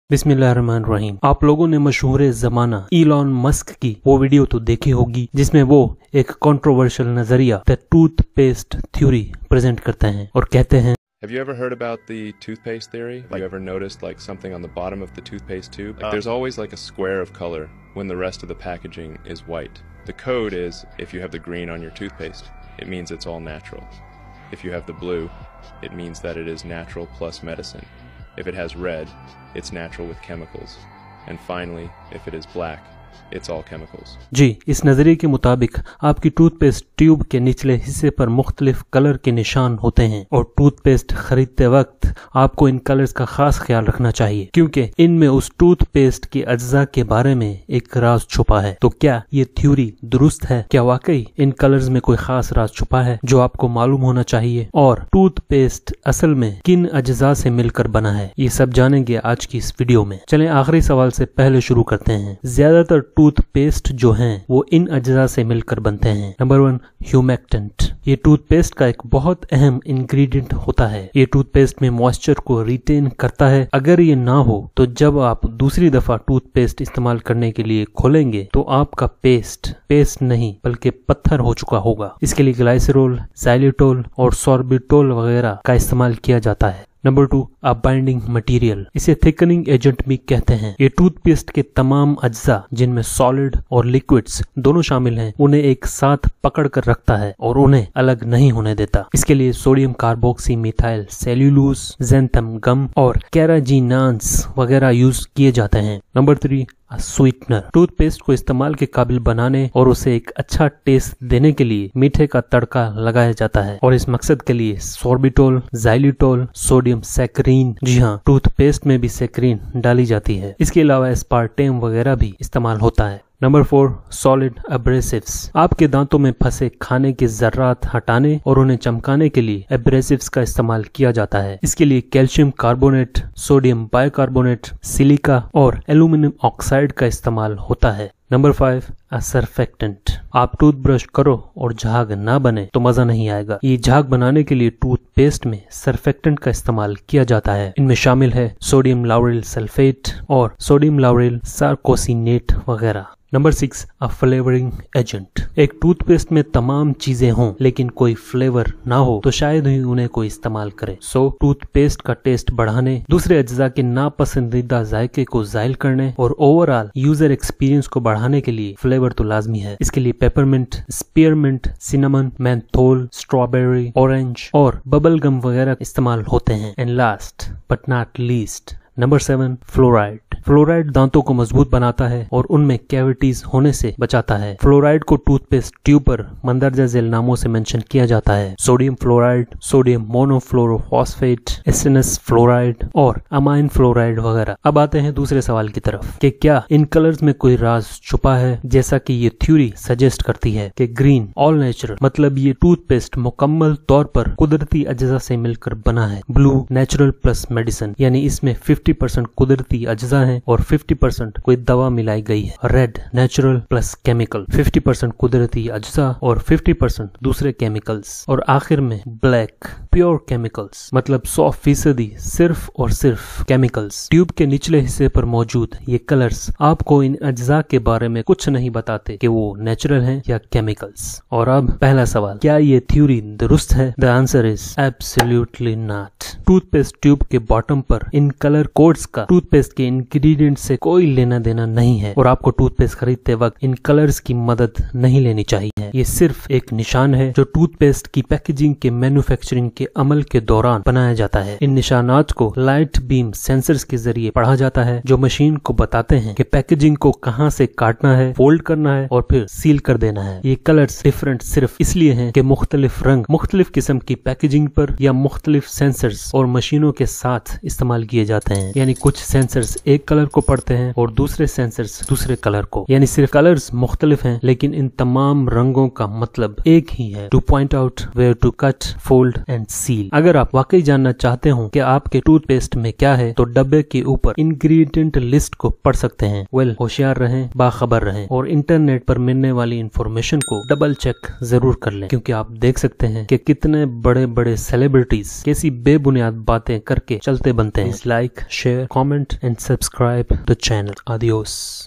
What Do the Colors on Toothpaste Tubes Really Mean?
Have you ever wondered what the colored rectangles at the bottom of your toothpaste tube mean? There's a common myth that these colors indicate the ingredients of the toothpaste. In this video, we debunk this myth and reveal the true purpose of these marks. You'll learn how these 'eye marks' play a crucial role in the manufacturing and packaging process. Join us as we separate fact from fiction and uncover the real story behind the colors on your toothpaste tube!